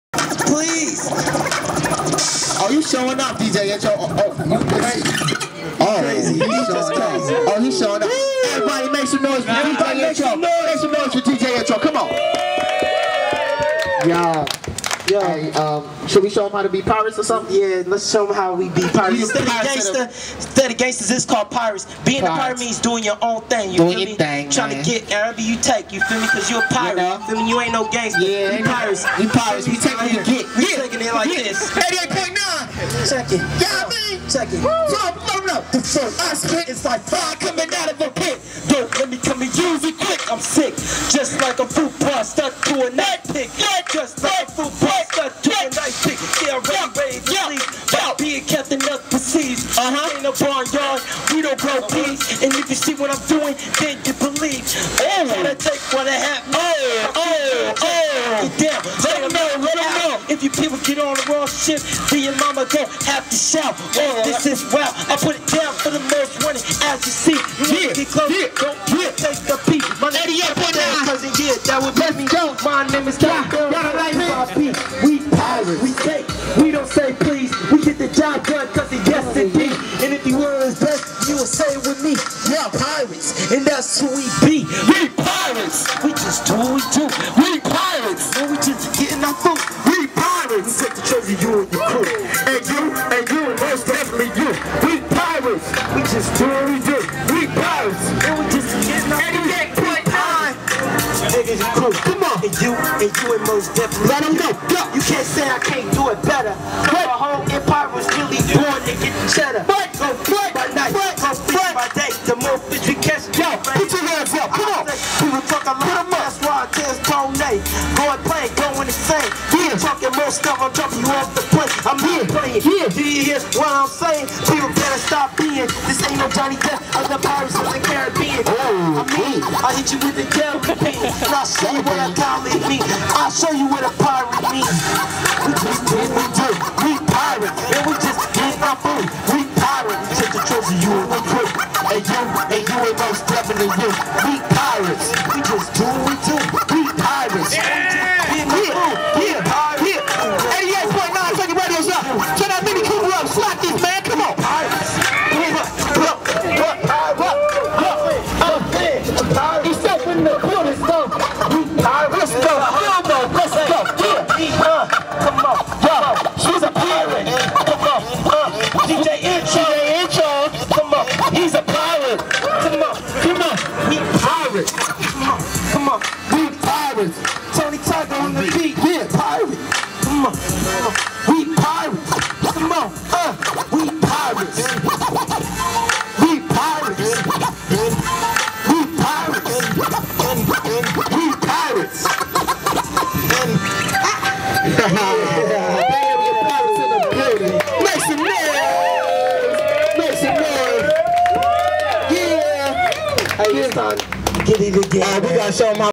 NPR BBQ 88.9 FM SAN FRANCISCO 2006
BLOCK PARTY IN THE MISSION DISTRICT, SAN FRANCISCO, FEATURING DJ INTRO, TONY JAZZO, FRANCOIS, NEIGHBORHOOD PUBLIC RADIO 88.9 FM